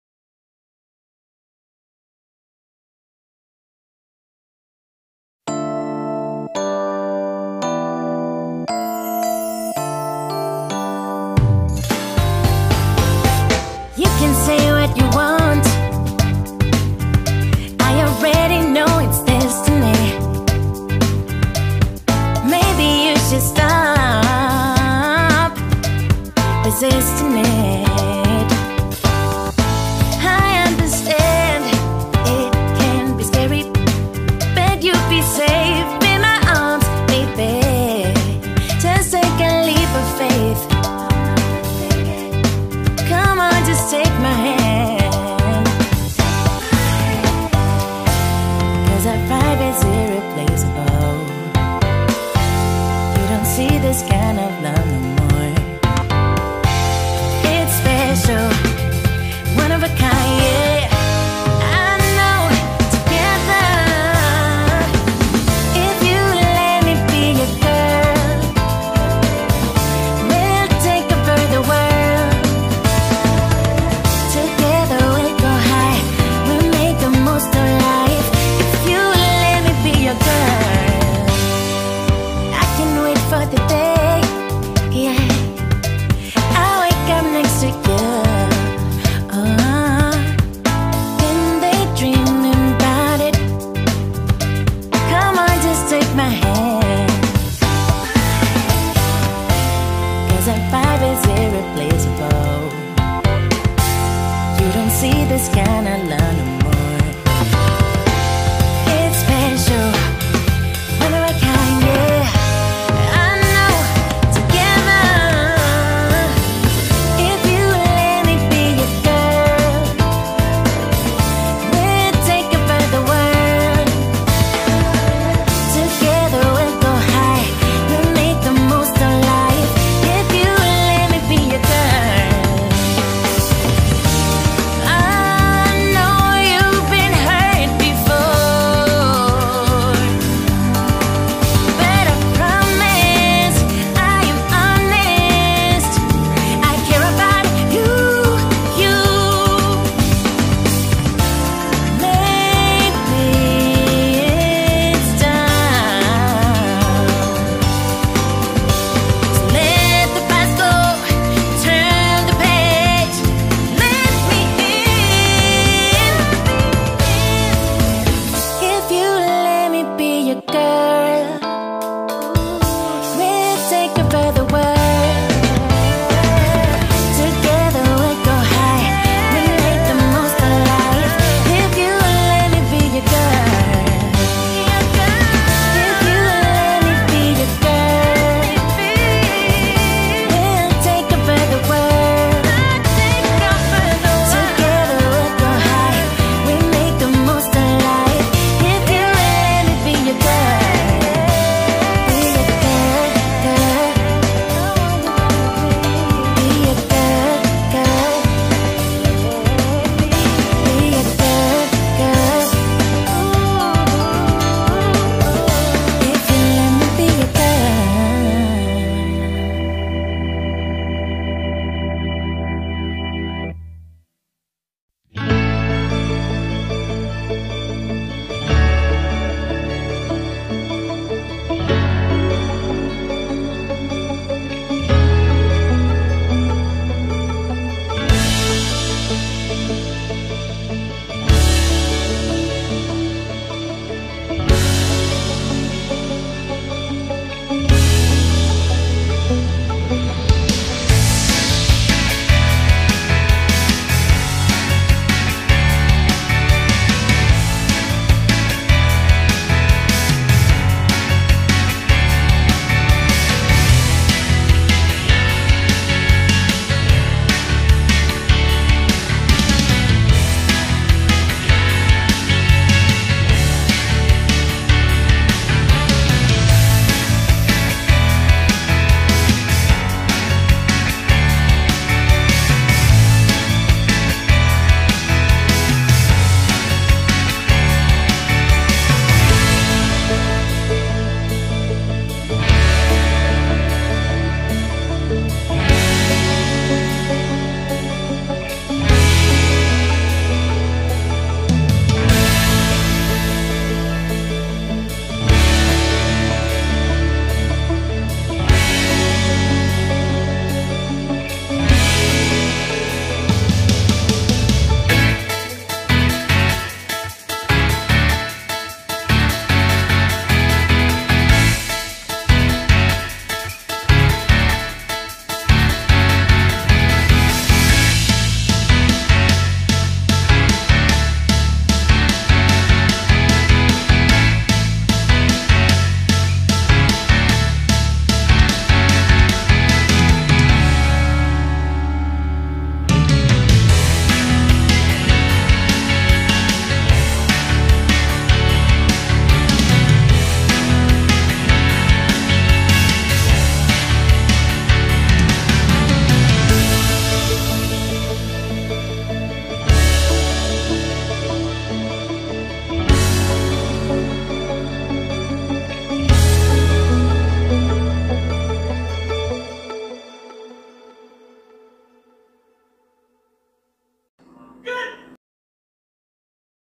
5월17일 우리좌대 낚시영상입니다!
참돔도 많이나오고! 미녀bj님도 오시고! 고기도 잘나오고!
오늘도 촬영에 협조해주셔서 감사합니다^^